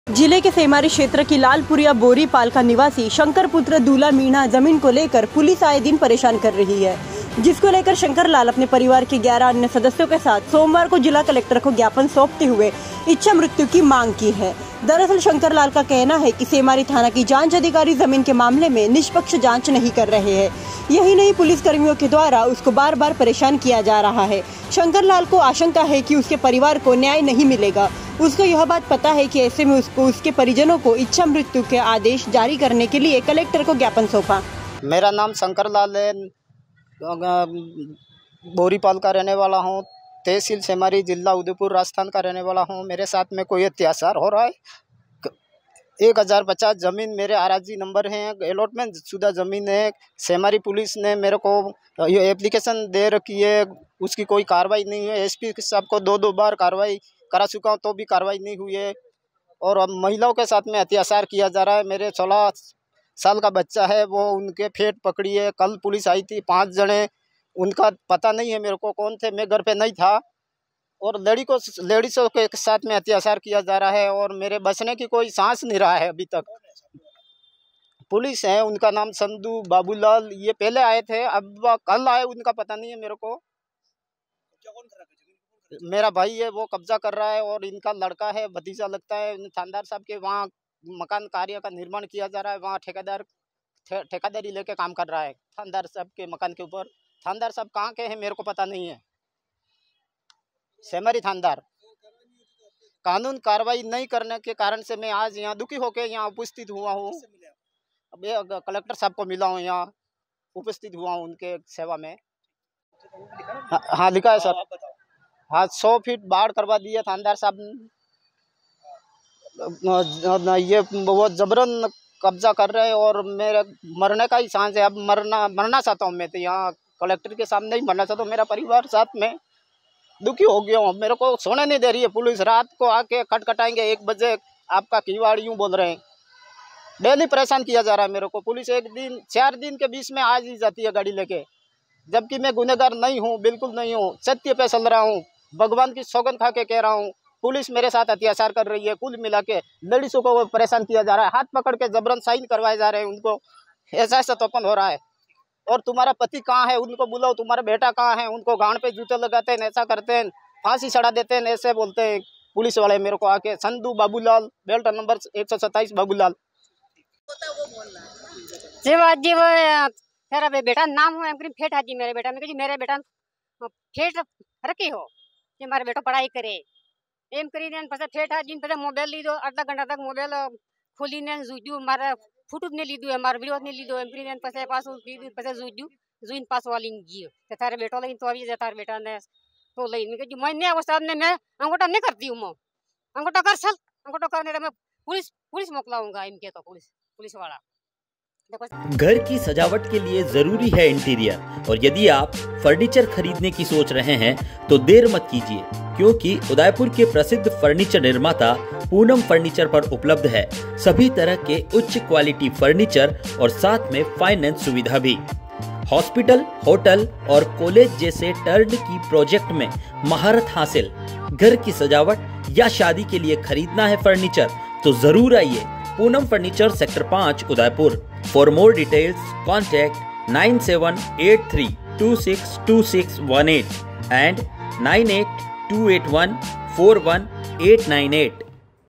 Udaipur पुलिस से परेशान शंकरलाल इच्छा मृत्यु की मांग को लेकर पहुचा कलेक्टर के पास !
पुलिस से परेशान शंकरलाल  इच्छा मृत्यु की मांग को लेकर पहुचा कलेक्टर के पास !